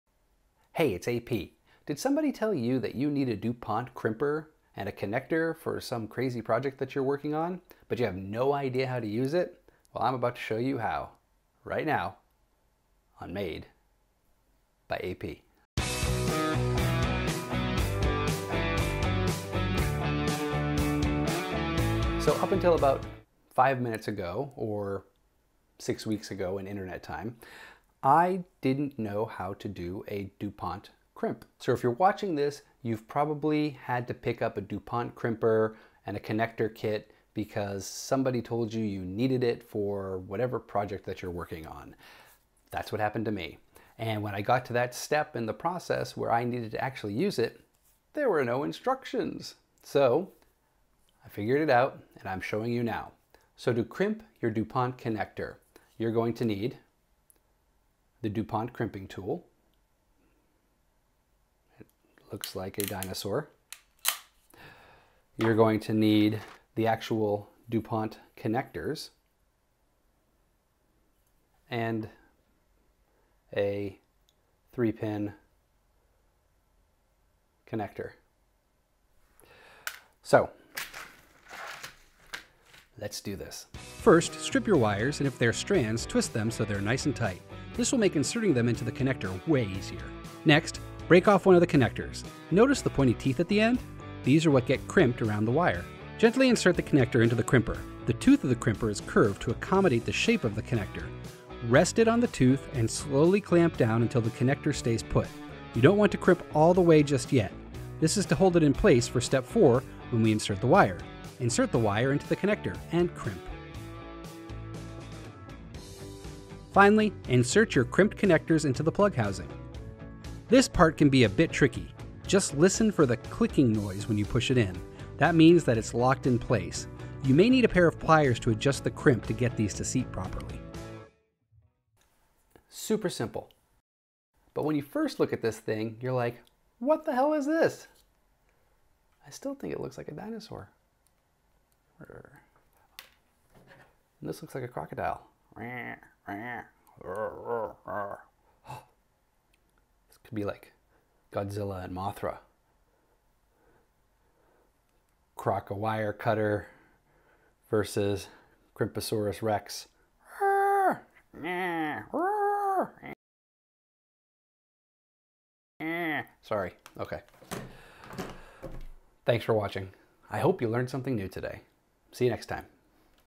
How to Make Dupont Connectors
So you want to now how to make Dupont Connectors (or less formally known as jumper cables)? Hopefully, this video will help you out! It’s super simple and you just need a few special tools like:

SN-025 Crimper
Wire Cutters/Strippers

Like what you see? Want to support future videos? Please consider shooting me a buck or two. Thanks!

** CHAPTERS **
How to make Dupont Connectors 00:00
What do you need to make a Dupont Connector 00:52
Make a Dupont Connector 01:22